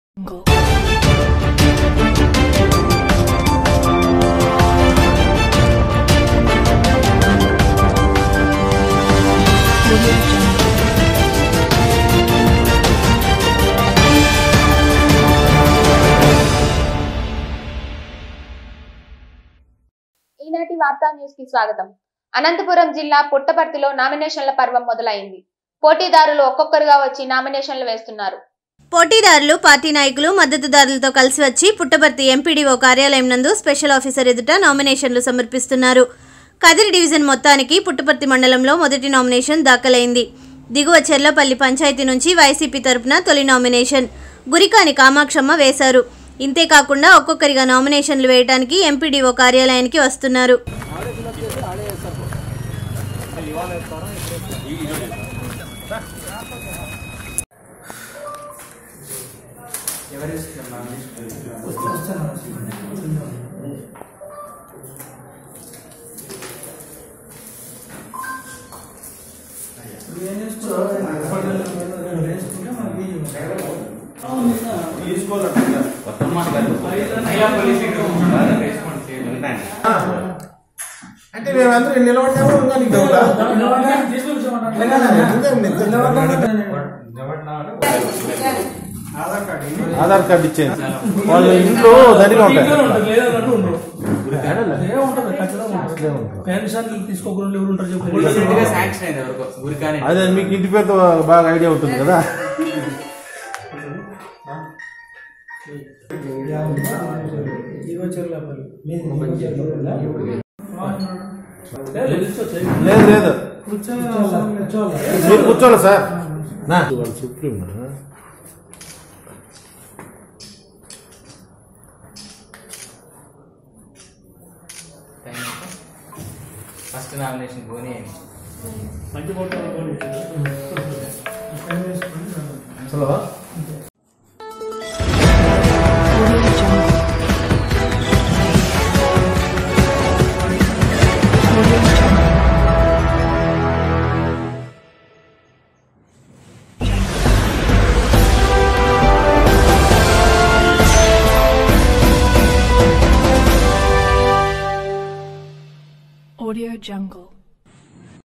అనంతపురం జిల్లా పుట్టపర్తిలో నామినేషన్ల పర్వం మొదలయింది.
పోటీదారులు ఒక్కొక్కరిగా వచ్చి నామినేషన్లు వేస్తున్నారు.